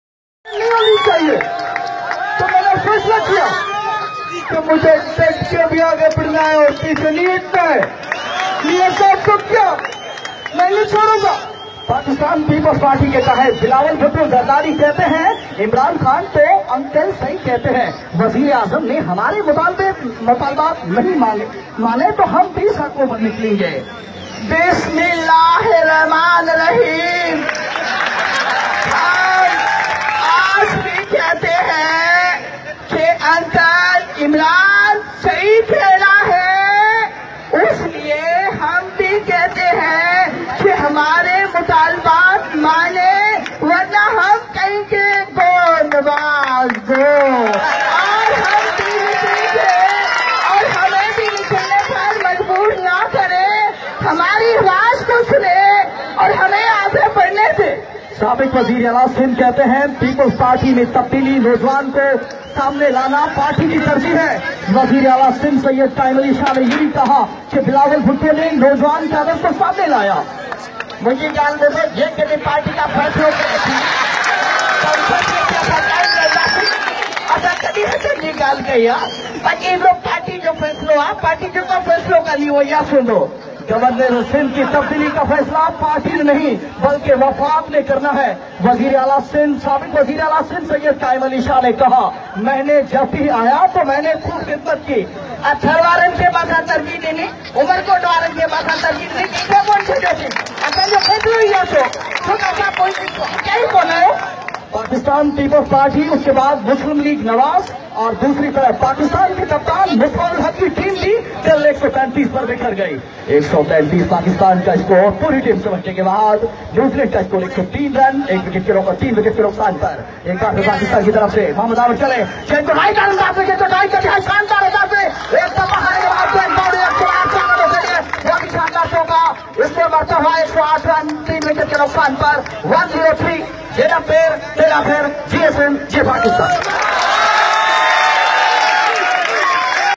pakistan politican's interstiing mimicry
This guy is from from newchor,Umerkot,Youth of Umerkot Organised first time a program in umerkot named"I am Umerkot Youth Festival" where youth showed their talent by participating in different activites,i found,this guy always keep intertaining to others to make smile on their faces,in this program he  performed pakistan politican's intersting Mimicry.